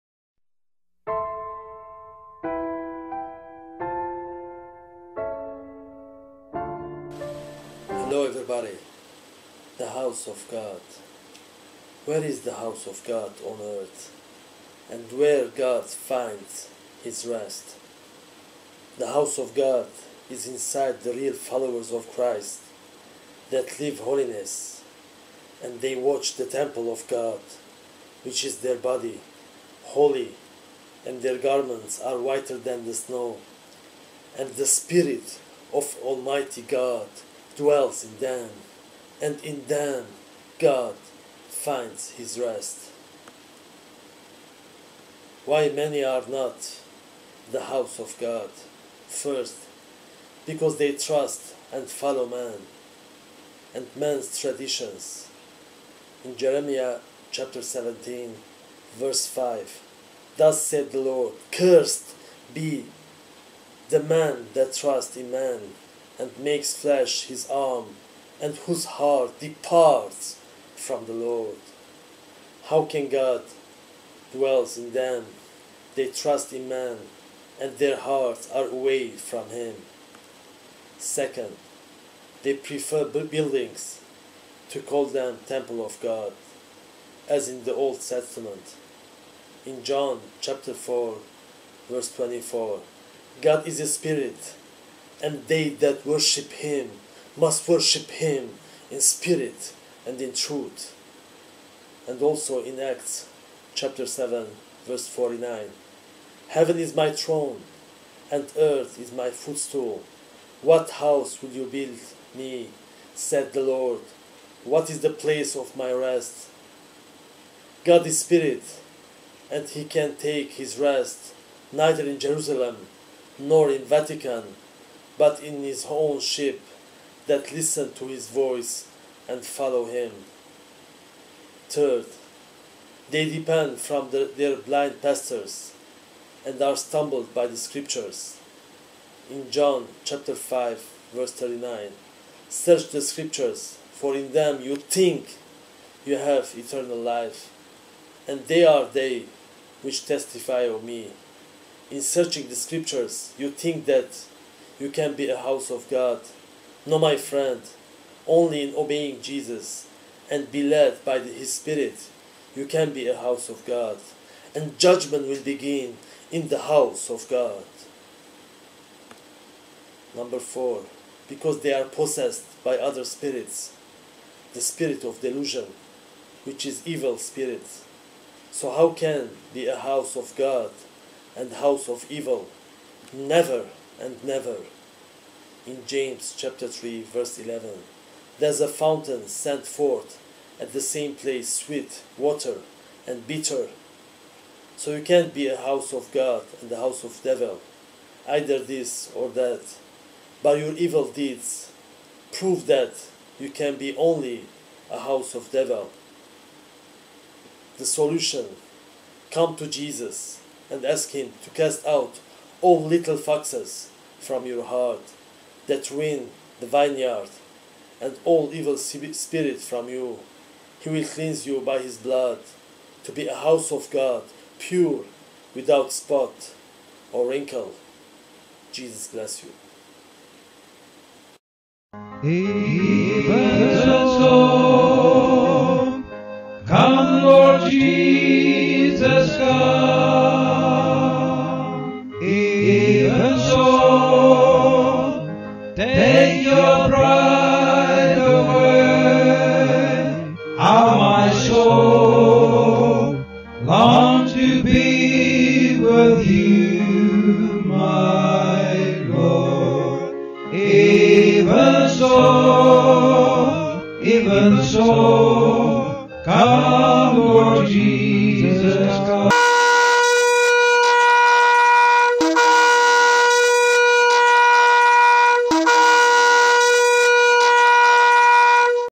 The house of God
Hello everybody, The house of God

Where's the house of God on earth? and where God finds His rest?

The House of God is inside the real followers of Christ, that live holiness and they watch the temple of God, which is their body, holy and their garments are whither than the snow, and the Spirit of almighty God dwells in them, and in them God finds His rest.

Why many are not the House of God?

1- Because they trust and follow men, and man's traditions, in Jeremiah 17:5 Thus said the LORD; Cursed be the man that trusts in man, and makes flesh his arm, and whose heart departs from the LORD, how can God dwells in them, they trust in man and their hearts are away from Him.

2 They prefer buildings, to call them temple of God, as in the old testament, in John 4:24 God is a Spirit: and they that worship him must worship him in spirit and in truth. and also in Acts 7:49 Heaven is my throne, and earth is my footstool: what house will you build me? said the Lord: or what is the place of my rest?, God is Spirit and He can take His rest neither in Jerusalem nor in vatican, but in His own sheep, that listen to His voice and follow Him.

3- They depend from their blind pastors, and are stumbled by the scriptures, in John 5:39 Search the scriptures; for in them you think you have eternal life: and they are they which testify of me, in searching the scriptures, you think that you can be a house of God, no my friend, only in obeying Jesus and be led by His Spirit, you can be a house of God, and judgment will begin in the house of God.

4- Because they are possessed by other spirits, the spirit of delusion, which is evil spirit, so how can be a house of God and house of evil, never & never. in James 3:11 Does a fountain send forth at the same place sweet water and bitter?, so you can't be a house of God and a house of devil, either this or that, but by your evil deeds prove that, you can be only a house of devil.

The solution: Come to Jesus and ask Him to cast out all little foxes from your heart, that ruin the vineyard, and all evil spirit from you, He will cleanse you by His blood, to be a house of God, pure without a spot or wrinkle.